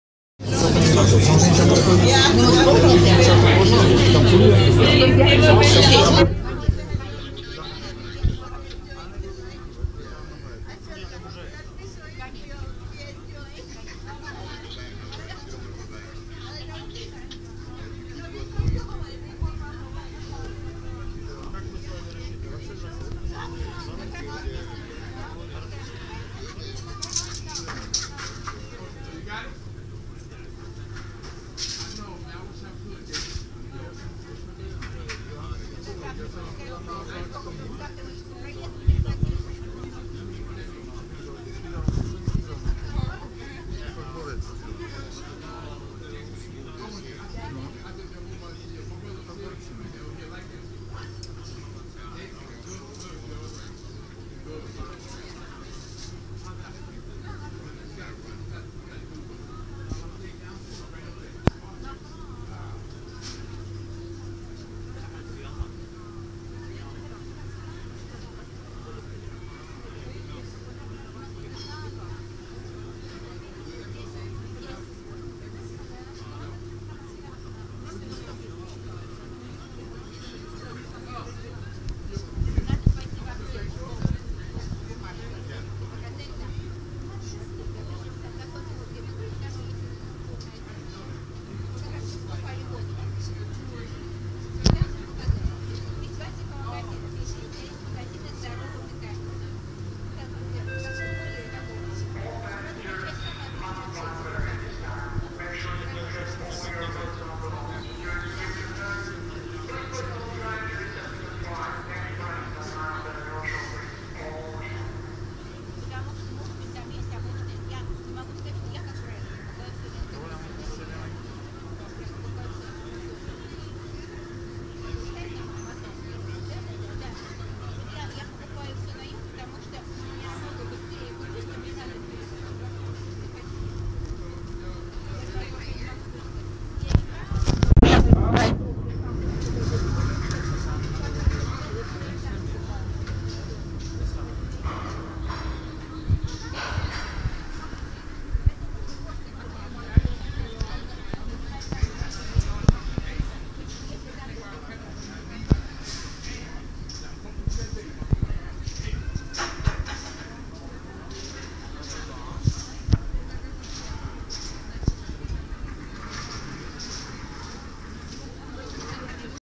Staten Island Ferry at South Ferry, Manhattan. July 26, 2015 PART 2.
Crossing the bay from Staten Island to Lower Manhattan -- South Ferry. Sunday evening, July 26, 2015
(see also: Part 1.)
Natural sound.
Videographed by Barbara Burke.